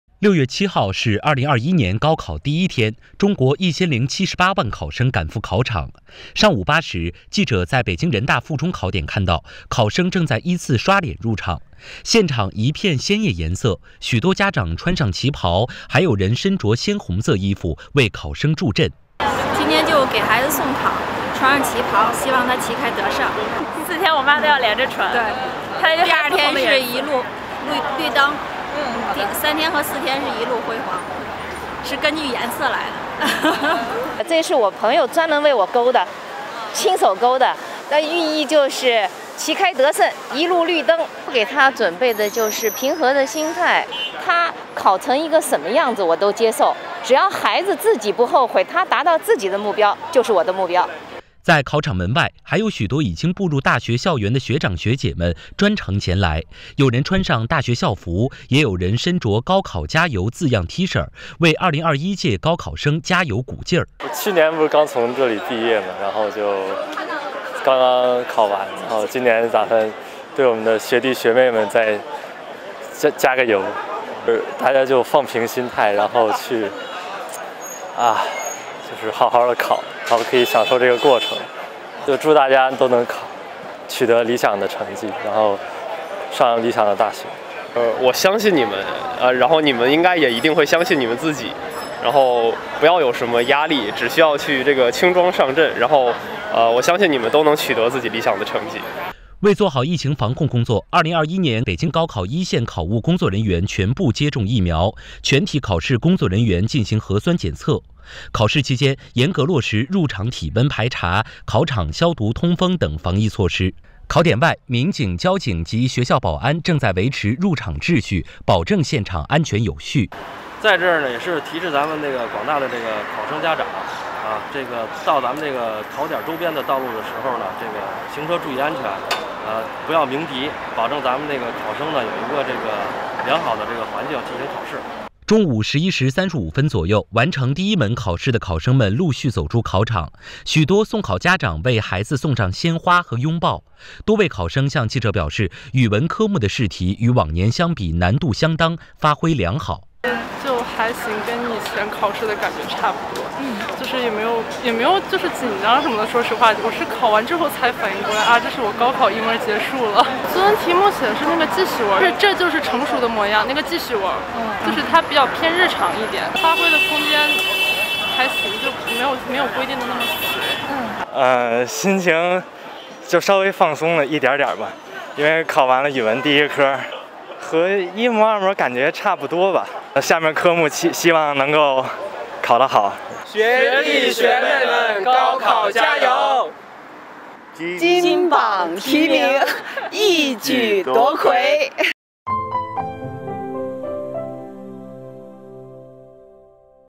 直击高考：家长旗袍助阵 清北学长来加油
6月7日是2021年高考第一天，中国1078万考生赶赴考场。上午8时，记者在北京人大附中考点看到，考生正在依次刷脸入场。现场一片鲜艳颜色，许多家长穿上旗袍，还有人身着鲜红色衣服，为考生助阵。